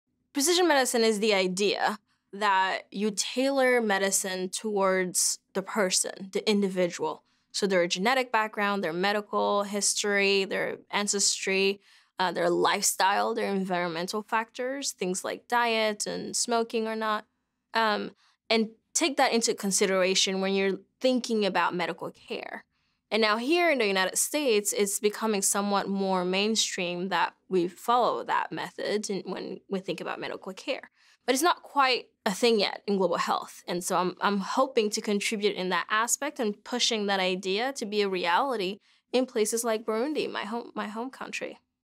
What is Precision Medicine | Biomedical Scientist Mireille Kamariza | Career Girls
Watch Mireille Kamariza, a Junior Fellow at Harvard University. She researches infectious diseases in the global health context.